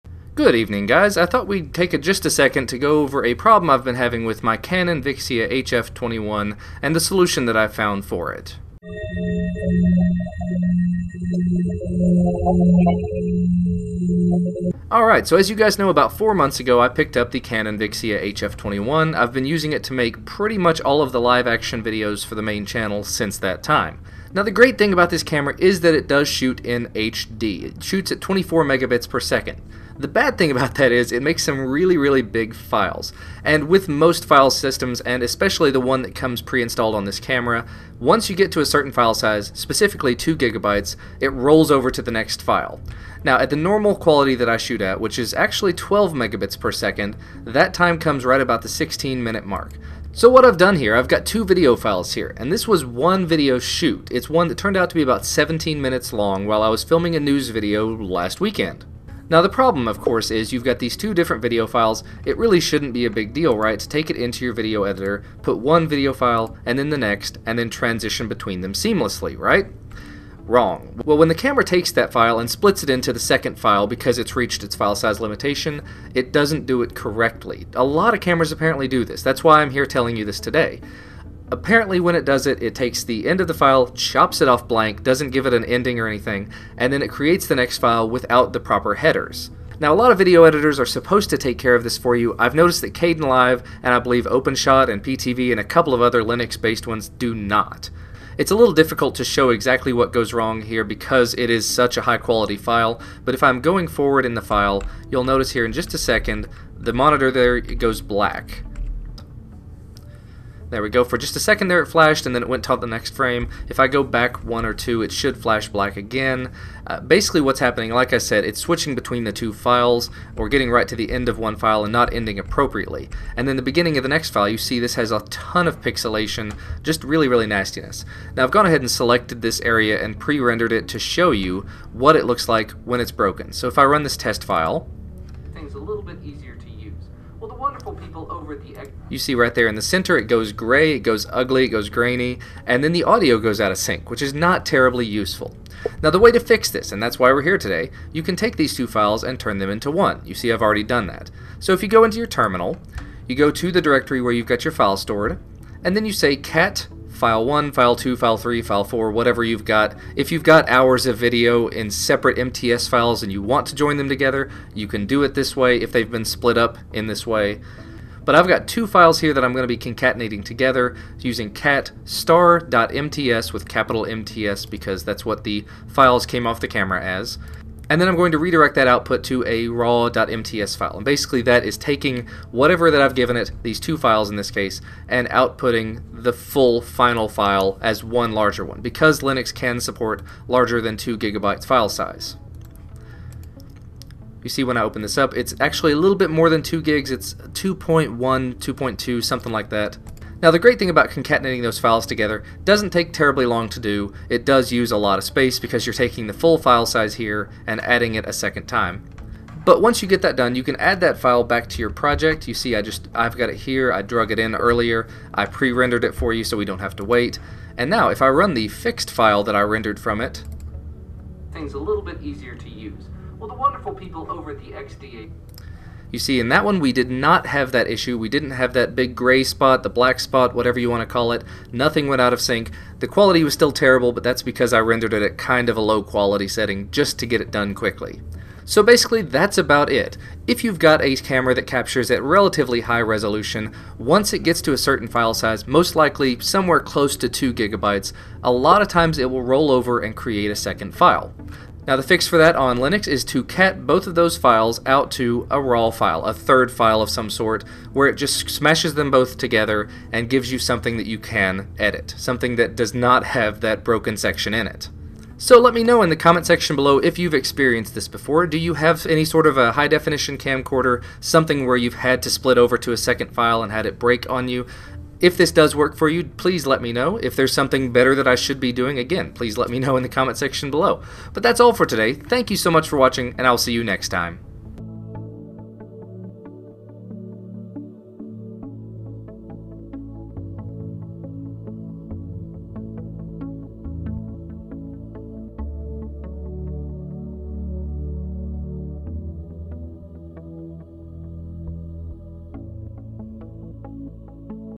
Fixing Split Video Files - Canon Vixia HF21 (and others)
// Source Code
Basically, if you have multiple files from your camera, you can go to the terminal and type
"cat *.FILETYPE &gt; file.FILETYPE"
(where FILETYPE is whatever type your camera uses. Mine uses .MTS)